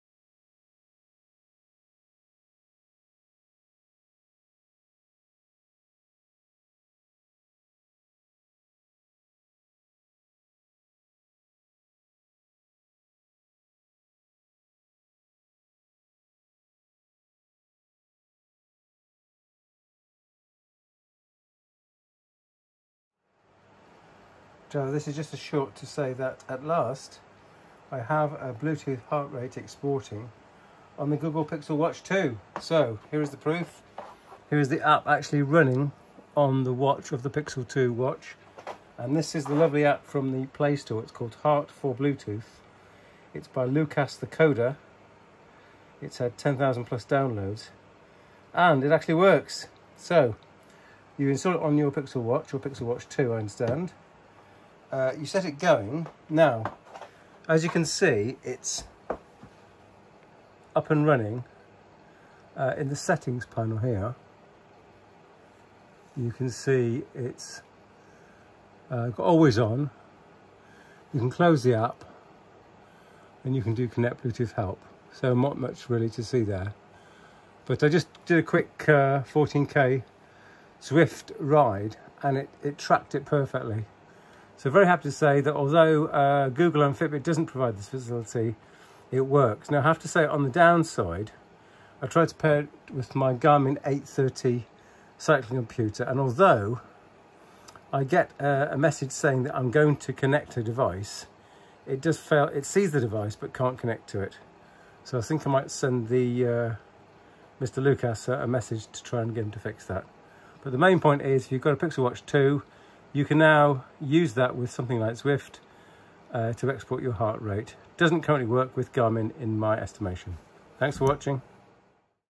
20231026 Google Pixel Watch 2 export HR Heart Rate over BTLE Bluetooth to Zwift
thanks to this android app

you can have HR export functionality.

If you follow the history of this on Reddit and other you will see this has been a request to Fitbit and now Google since about 2018.

But they never seemingly be bothered to provide this rather obvious and often asked for functionality.

So very pleased!

Currently (20231026) I tried and failed to make a connection from the Pixel Watch 2 to a Garmin 830 cycle computer.  the Garmin sees the HR Pixel 2 device but cannot seem to connect.